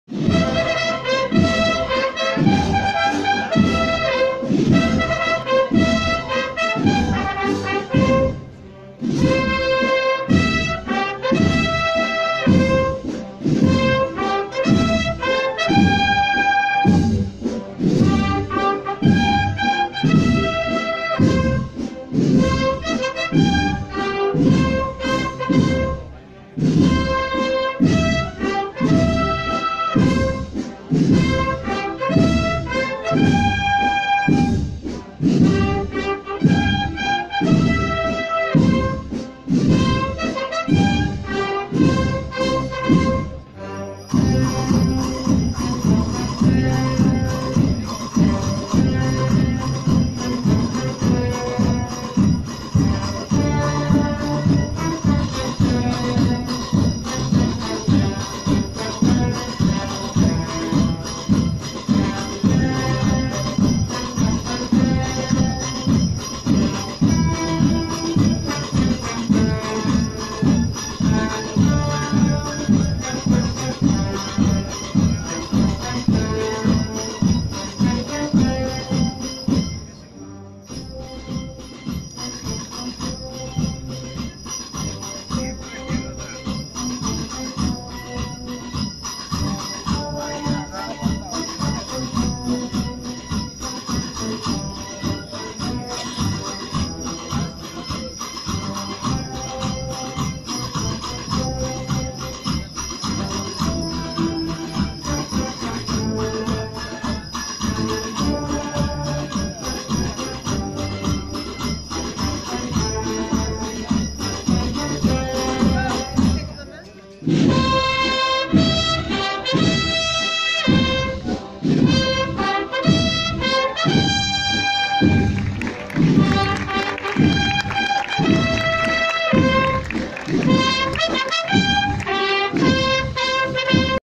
VIRGEN DE LA AURORA EN LA CAPILLA DE MONTESION. MAÑANA DEL DOMINGO DE RESURRECCIÓN DE 2019 SEVILLA
Virgen de la Aurora, de la Hermandad de la Resurrección, en el saludo a la Hermandad de Montesion en la calle Feria de Sevilla. Mañana del domingo de Resurrección de 2019